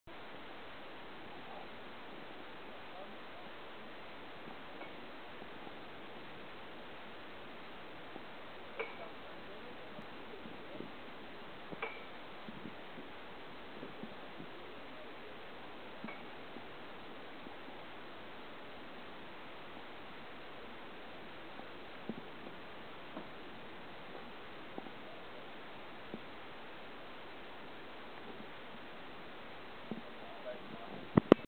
View from the Wank
A 180 degrees shot from the Wankhaus near the summit of Wank. Looking down to Garmisch-Partenkirchen and covering the Wettersteingebirge from West to East on 27th February 2010